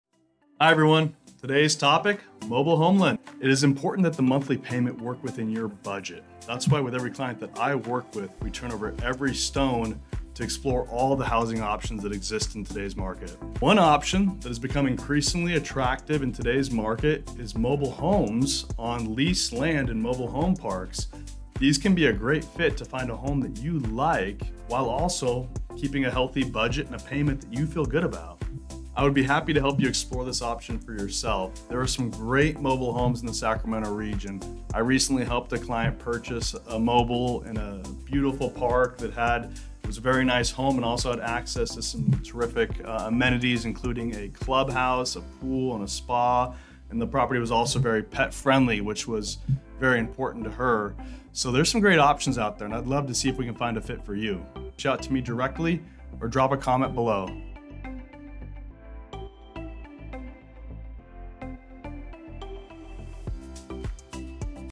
Mobile Financing Alex
In today's challenging real estate market we need to think outside the box, enter mobile home loans!